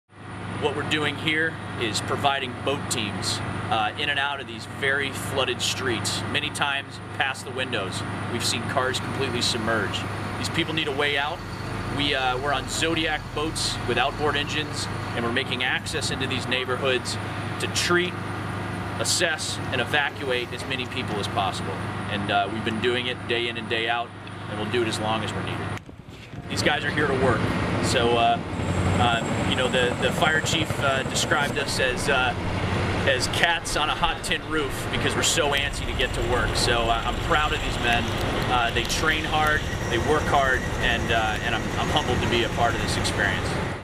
National Guard members continue Harvey relief operations throughout Texas
The National Guard continues Hurricane Harvey response efforts assisting local authorities with search and rescue and evacuation of areas affected by flooding.  (Video by Maj. Randall Stillinger and
Staff Sgt. Balinda O'Neal Dresel, Edited by Tech. Sgt. Steven Schwach)